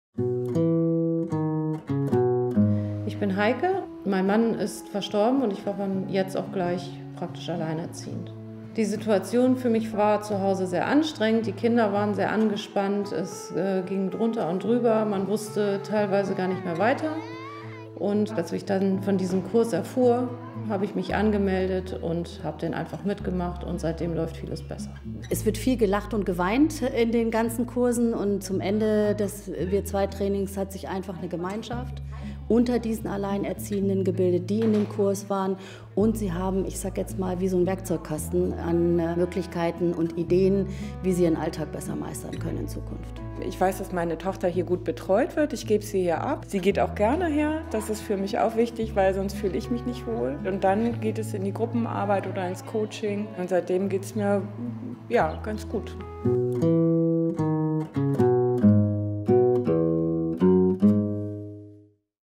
Alleinerziehend, aber nicht allein: Das Projekt "Wir2"
In Ratzeburg können Alleinerziehende im Projekt "Wir2" Austausch und Hilfe finden.